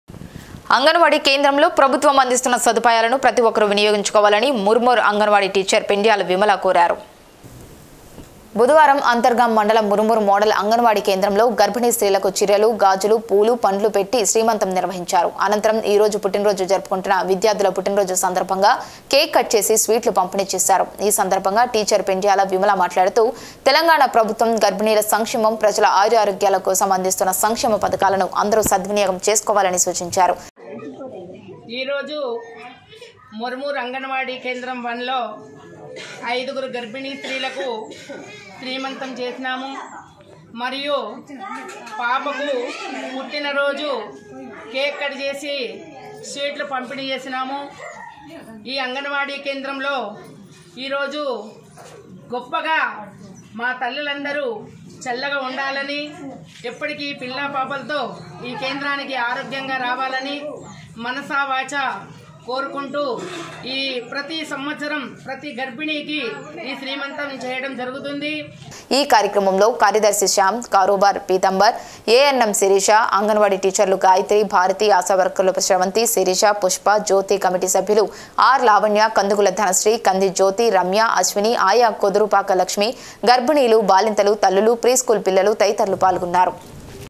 ప్రభుత్వం అందిస్తున్న సదుపాయాలను ప్రతిఒక్కరూ వినియోగించుకోవాలి-- అంగన్వాడీ టీచర్ పెండ్యాల విమల
SITI CABLE GTPL GDK.